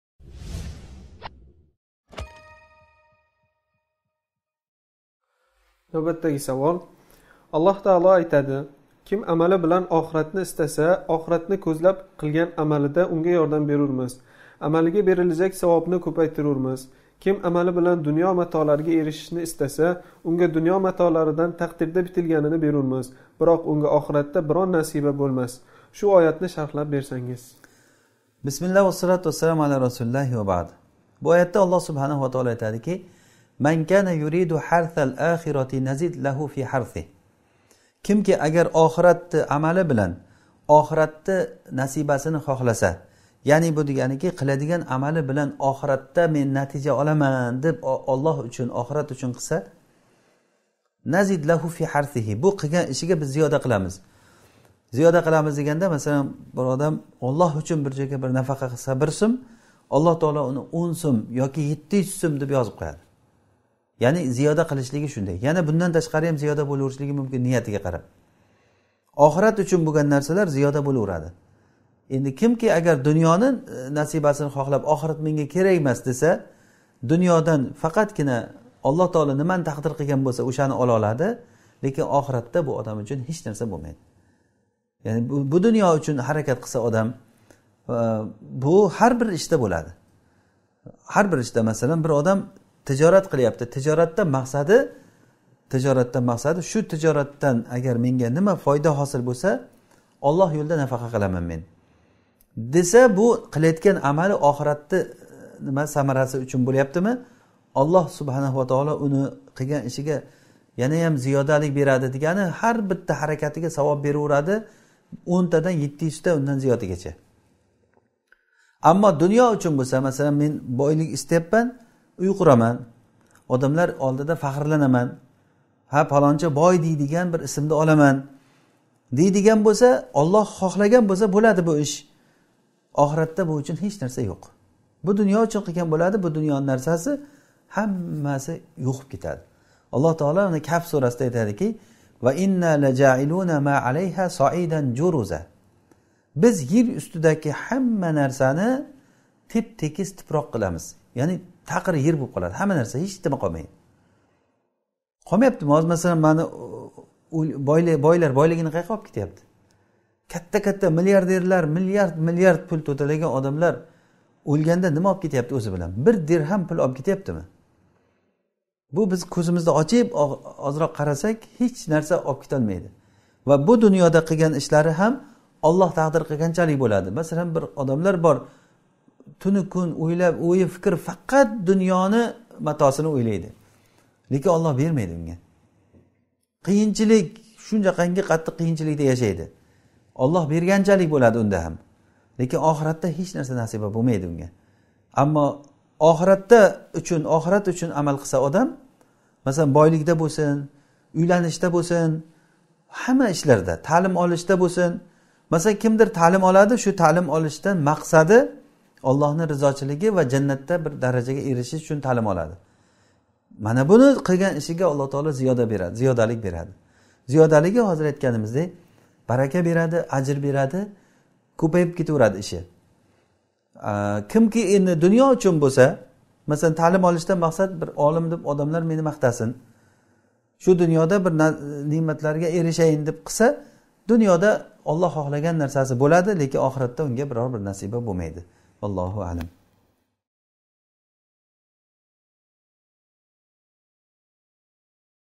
Sho‘ro surasi, 20-oyat tafsiri | Shayx Sodiq Samarqandiy
Alloh taolo aytadi: «Kim amali bilan oxiratni istasa, oxiratni ko‘zlab qilgan amalida unga yordam berurmiz, amaliga berilajak savobni ko‘paytirurmiz. Kim amali bilan dunyo matolariga erishishni istasa, unga dunyo matolaridan taqdirda bitilganini berurmiz. Biroq unga oxiratda biron nasiba bo‘lmas» [Sho‘ro: 20]. Shu oyatni sharhlab bersangiz.